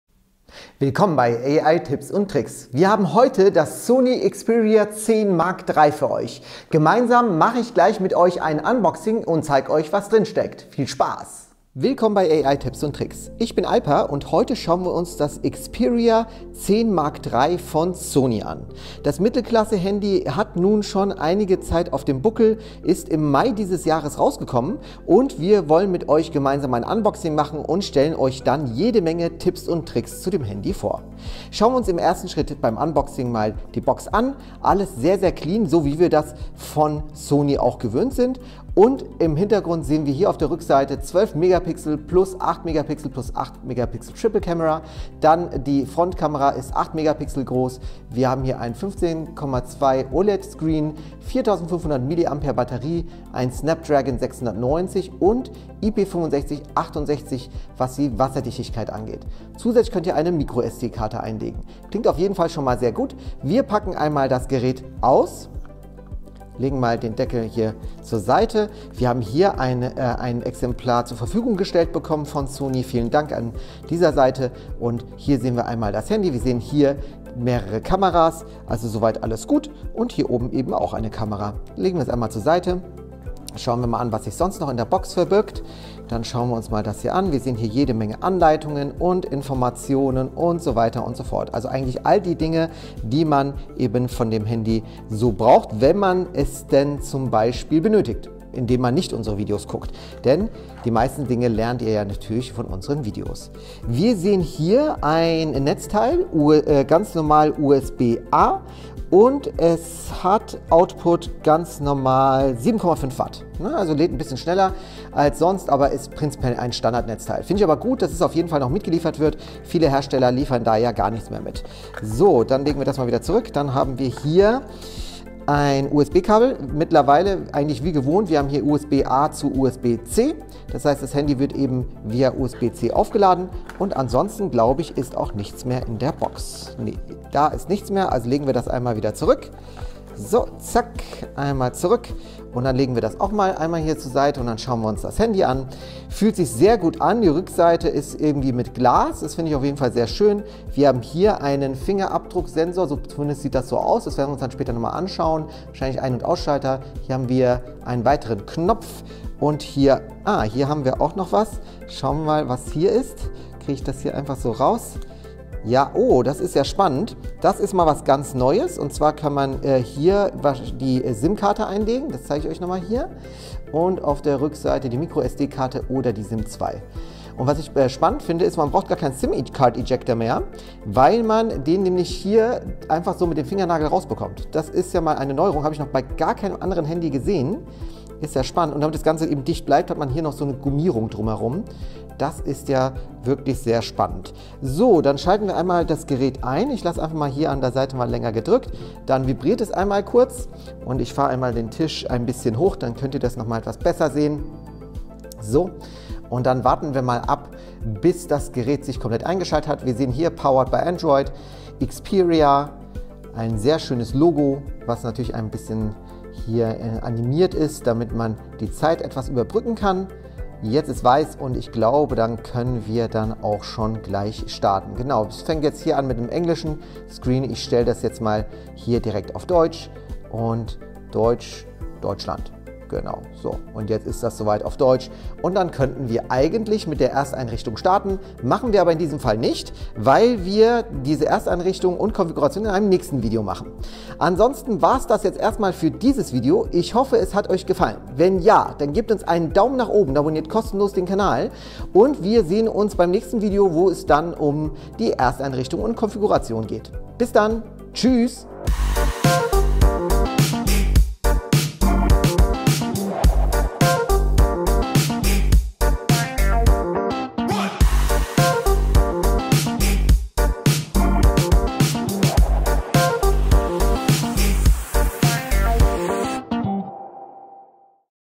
SONY Xperia 10 III - Unboxing (deutsch) • 📱 • 📦 • 🆕 •
Ihr interessiert Euch für das SONY Xperia 10 III und wollt Euch im Unboxing einen ersten Eindruck verschaffen? Wir leiten Euch sehr gerne durch das Unboxing vom Xperia 10 III und versuchen, Euch einen möglichst guten Ersteindruck zu geben. Sehen wir uns doch mal an, wie die Verpackung designed ist und was noch alles im Lieferumfang enthalten ist. Natürlich schauen wir uns auch das Xperia 10 III an sich einmal an. Viel Spaß.